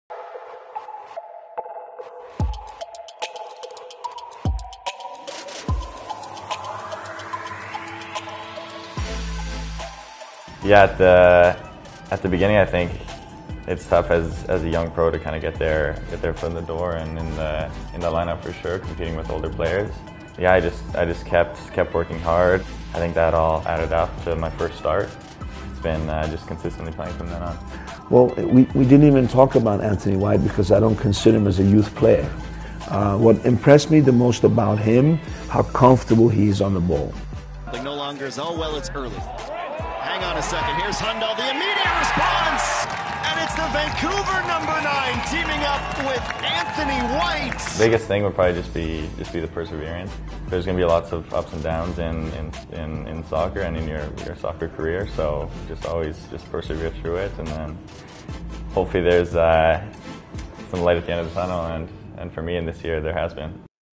NextGen, presented by Jones DesLauriers | Navacord: Anthony White
The third player named to the CPL NextGen class of 2023, presented by Jones DesLauriers | Navacord, is Vancouver FC defender Anthony White  🦅